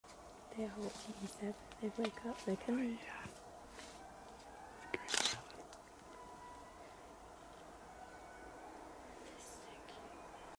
Most Adorable Lemurs Ever | Madagascar
This pair of Eastern Woolly Lemurs were just settling down for some sleep in the early morning hours in Mitsinjo National Park, within the Anamalazaotra Forest in eastern Madagascar.  They are nocturnal creatures.  Sooo incredibly cute!